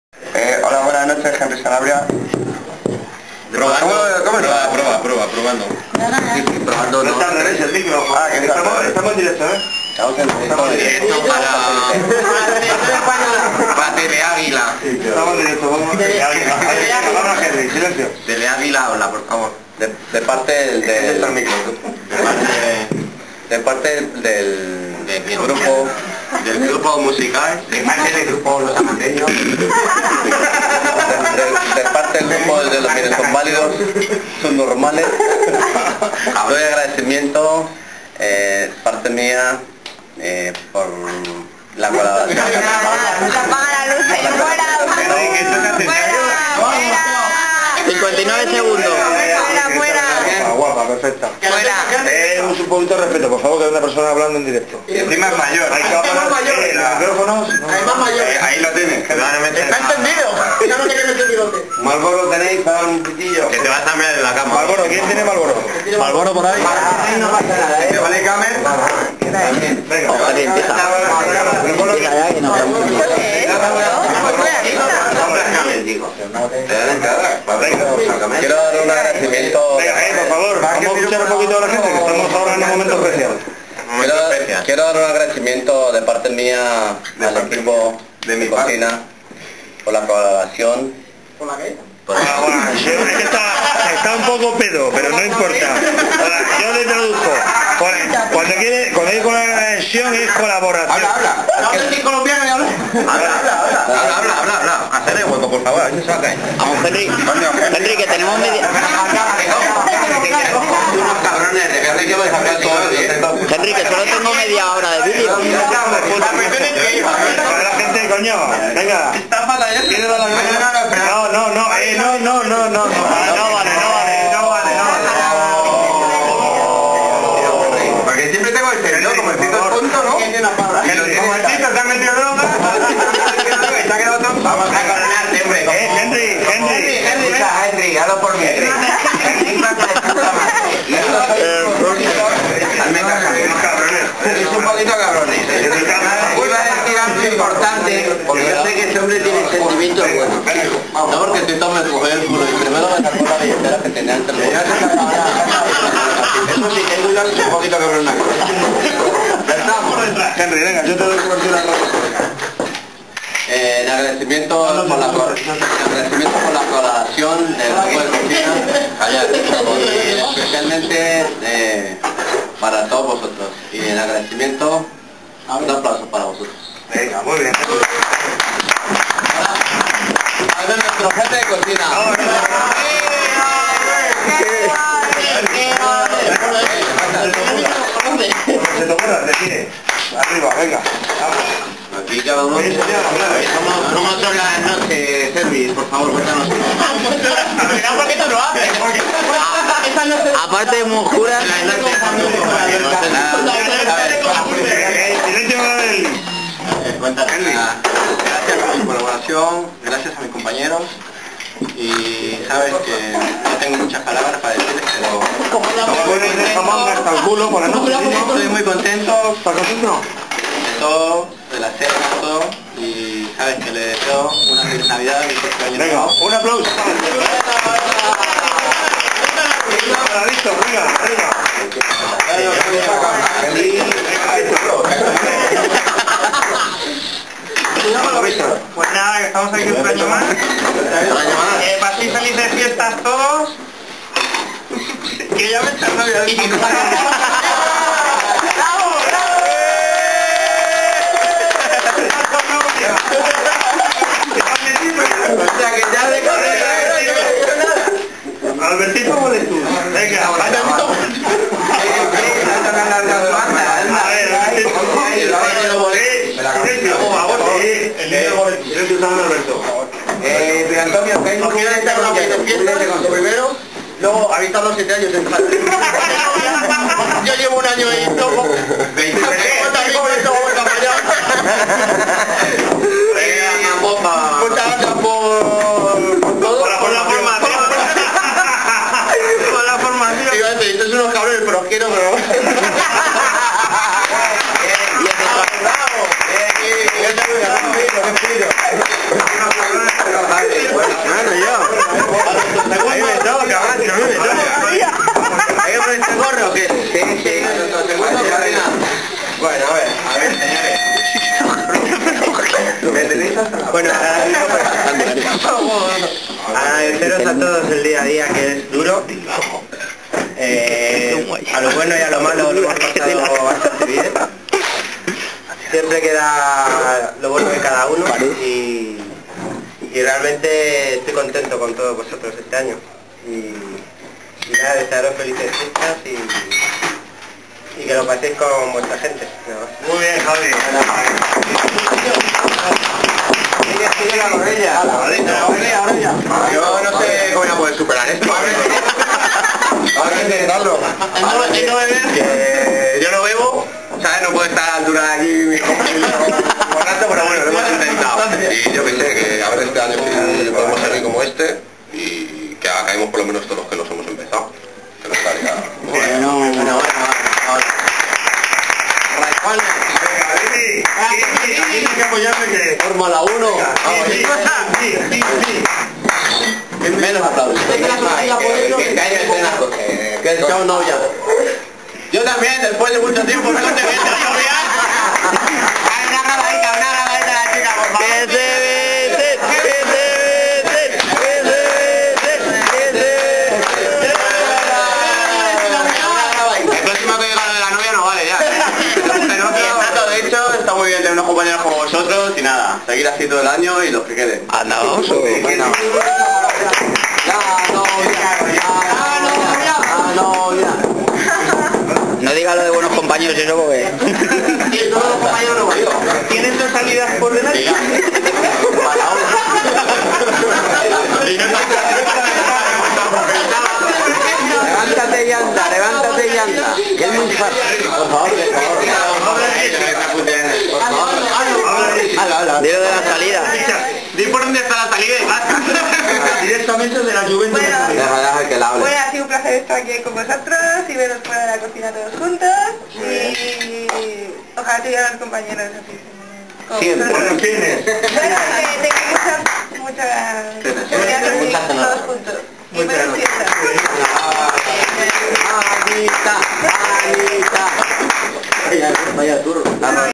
Cena Cocina Pullman 2008 (1parte)
Después de la cena...unas palabritas...